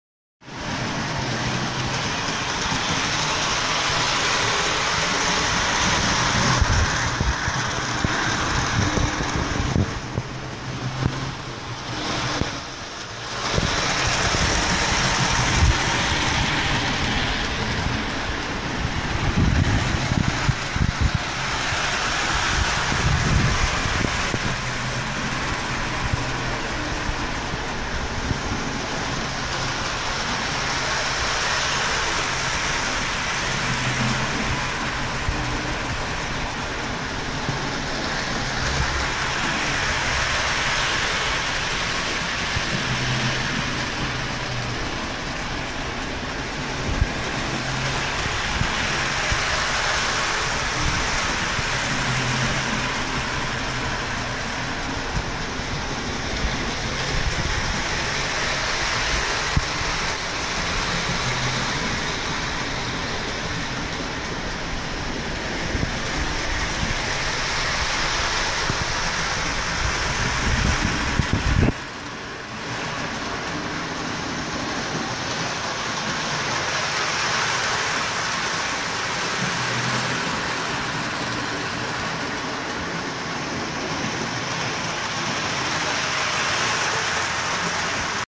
Merry Christmas Village
A visit to a RMLI Member brings us a video experience of their annual Christmas Putz with a Lionel Polar Express outfit front and center.  Happy holiday memories!